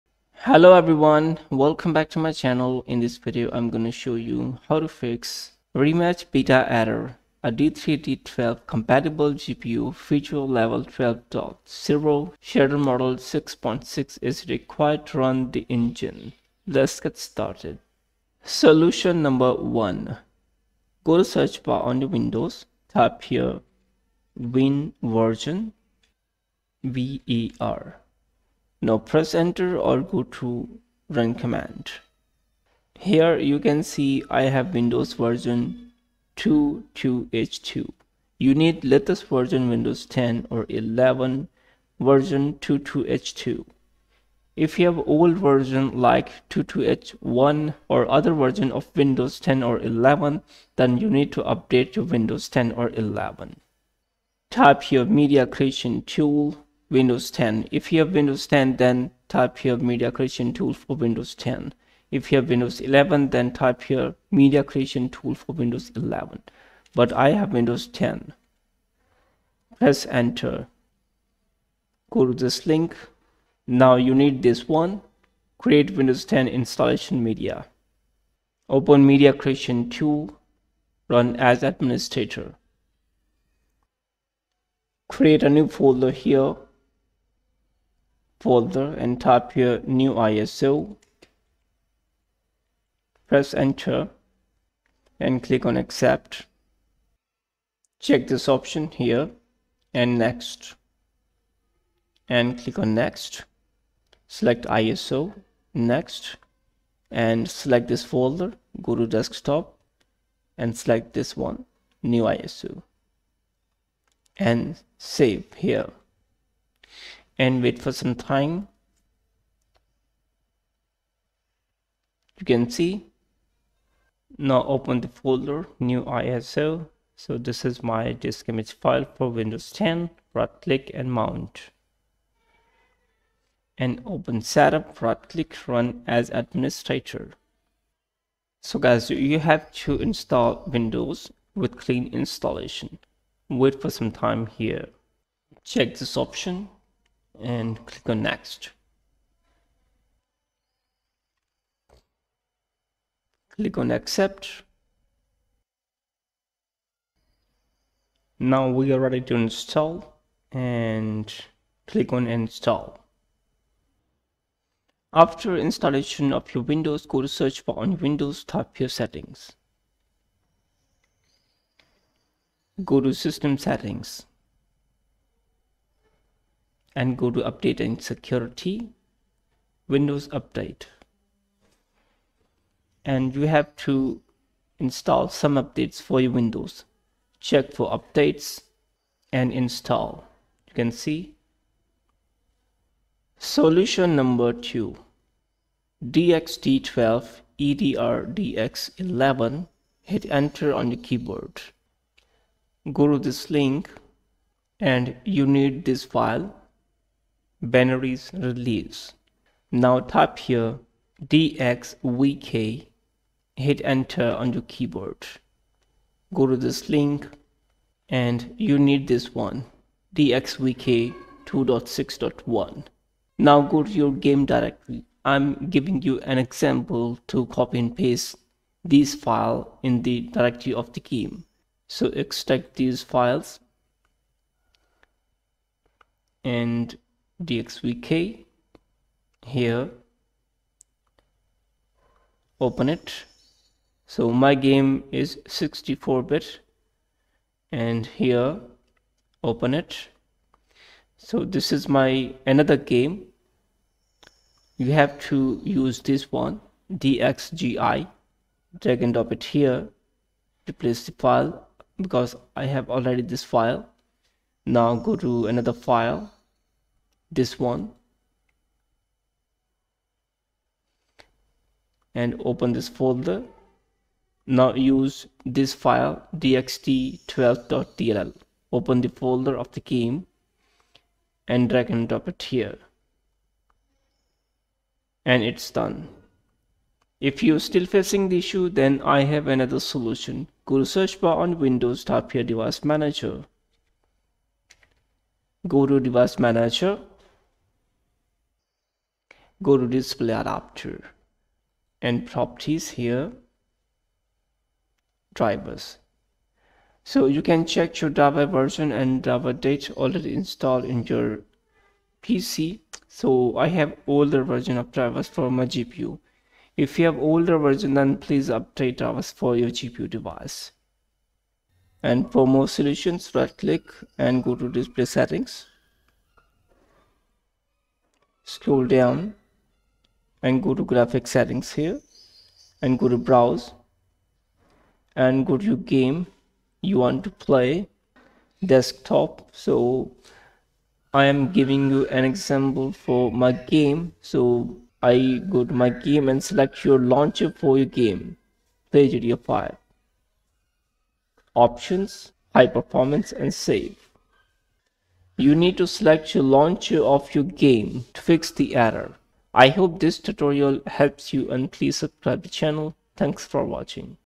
Rematch beta error A D3D12 Compatible GPU (Feature level 12.0, Shader Model 6.6) is required Fix
Hello Guys,
I have shown in this video Rematch beta error A D3D12 Compatible GPU (Feature level 12.0, Shader Model 6.6) is required Fix.